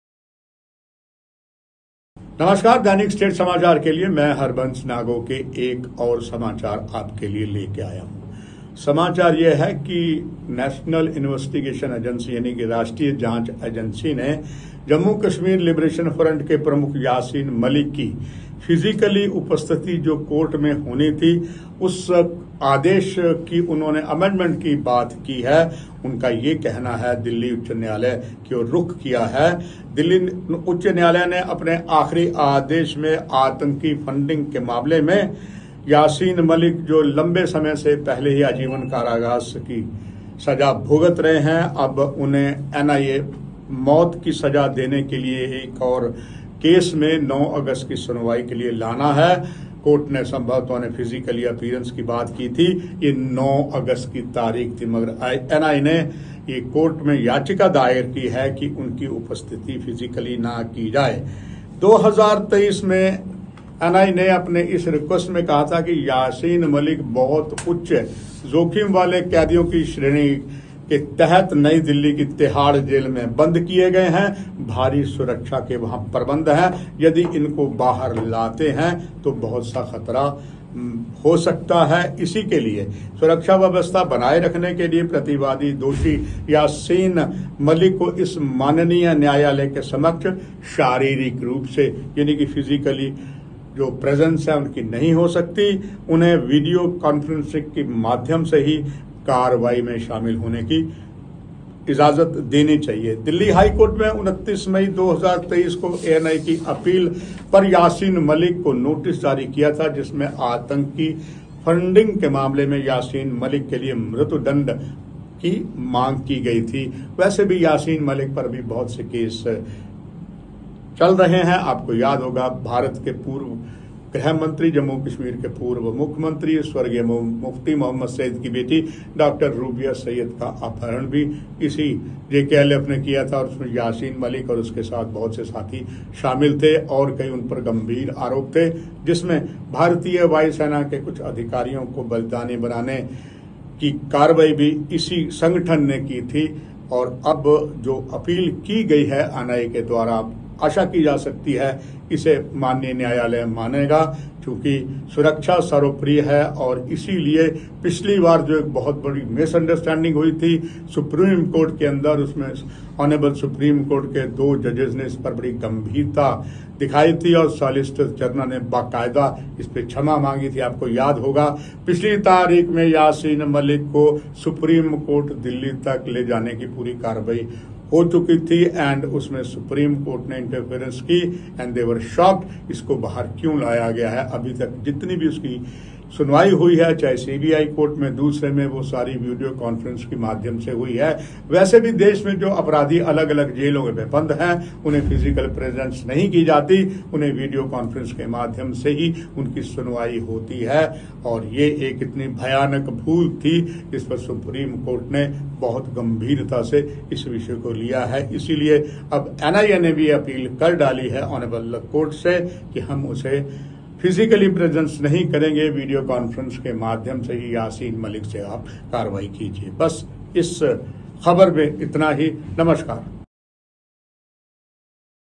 वीडियो कॉन्फ्रेंसिंग के जरिए पेशी की मांग को लेकर हाईकोर्ट पहुंची NIA, सुनवाई सात अगस्त को
भारी सुरक्षा मुद्दे का हवाला देते हुए राष्ट्रीय जांच एजेंसी (एनआईए) ने कश्मीरी अलगाववादी नेता यासीन मलिक को तिहाड़ जेल से वीडियो कॉन्फ्रेंसिंग के माध्यम से पेश करने का निर्देश देने की मांग को लेकर दिल्ली हाईकोर्ट का रुख किया है। एनआईए ने नौ अगस्त को व्यक्तिगत रूप से पेश करने के अदालत के आदेश में संशोधन की मांग करते हुए आवेदन दायर किया है। अपरिहार्य कारणों से बृहस्पतिवार को न्यायमूर्ति सिद्धार्थ मृदुल की अध्यक्षता वाली पीठ के न बैठने के कारण सुनवाई सात अगस्त के लिए स्थगित कर दी गई।